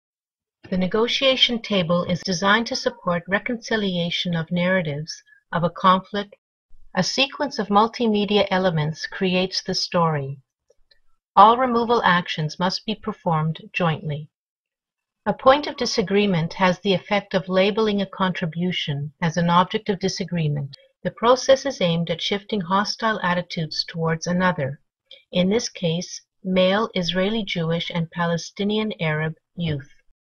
Co-narrating a conflict: An interactive tabletop to facilitate attitudinal shifts
Authors:

Massimo Zancanaro, Oliviero Stock, Zvi Eisikovits, Chaya Koren, Patrice L. Weiss

Abstract:

A multi-user tabletop interface was designed to support reconciliation of a conflict aimed at shifting hostile attitudes and achieving a greater understanding of another viewpoint. The interface provided a setting for face-to-face shared narration and support for the management of disagreements. The interface allows for escalation and de-escalation of the conflict emerging in the shared narration and requires that participants perform joint actions when a contribution to the story is to be removed from the overall narration. A between-subjects experiment compared the tabletop interface and a desktop multimedia interface with mixed pairs (male Israeli-Jewish and Palestinian-Arab youth). The results demonstrated that the experience with the tabletop interface appears to be motivating and, most importantly, produces at least a short-term shift of attitude toward the other.